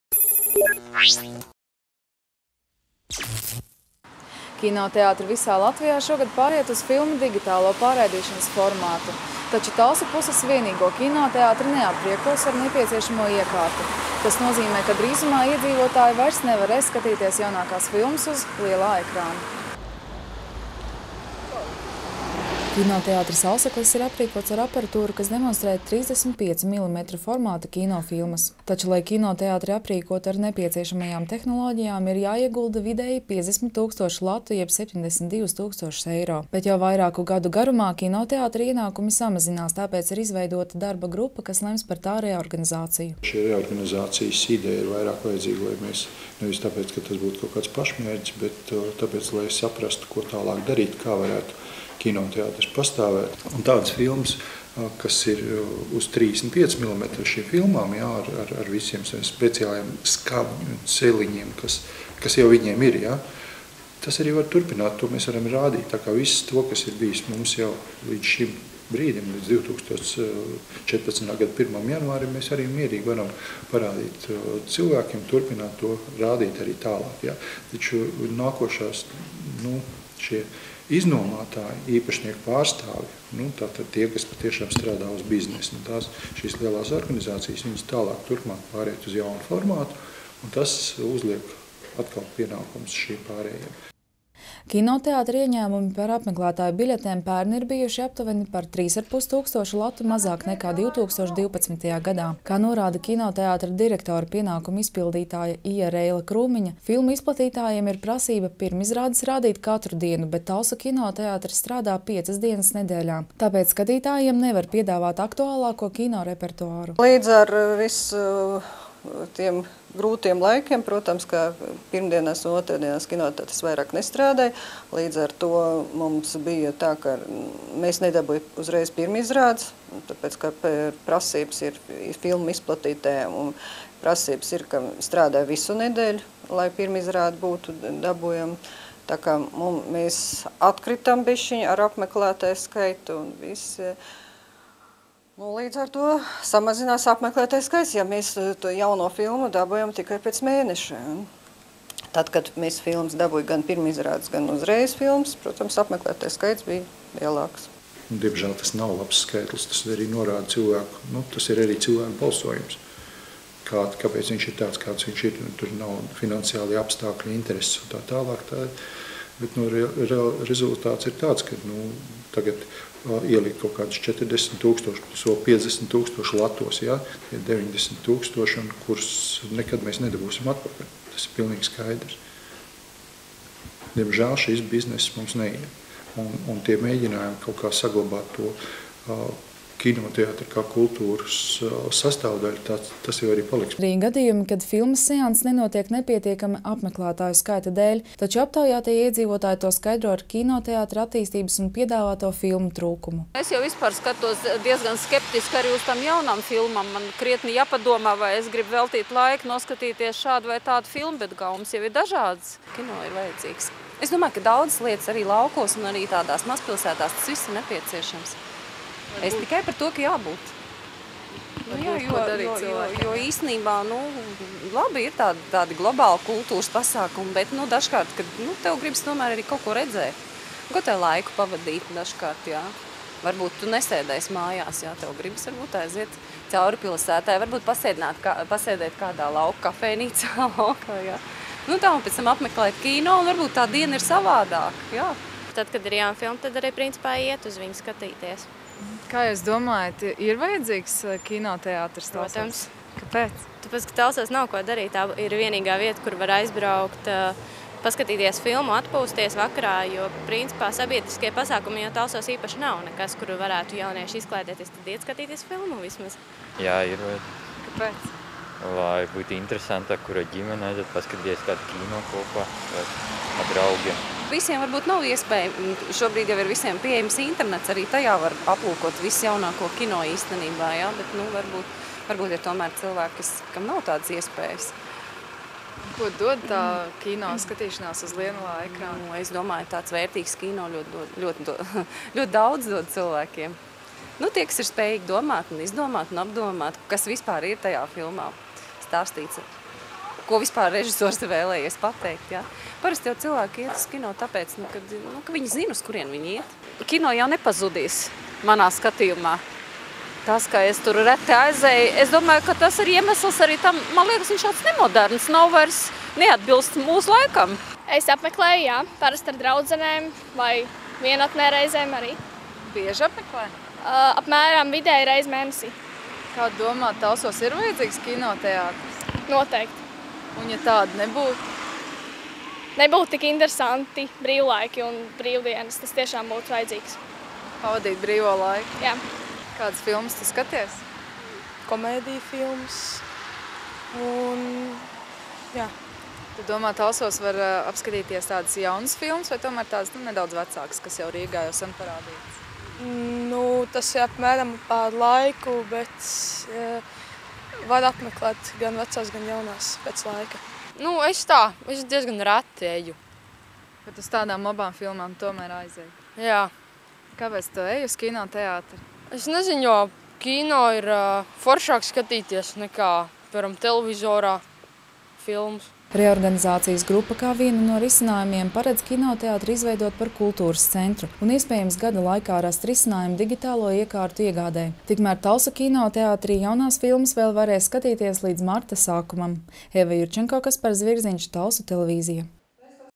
Reorganizēs kinoteātri "Auseklis"
Kinoteātri visā Latvijā šogad pāriet uz filmu digitālo pārraidīšanas formātu. Taču Talsu puses vienīgo kinotēatri neaprīkos ar nepieciešamo iekārtu. Tas nozīmē, ka drīzumā iedzīvotāji vairs nevarēs skatīties jaunākās filmas uz kinoteātra lielā ekrāna.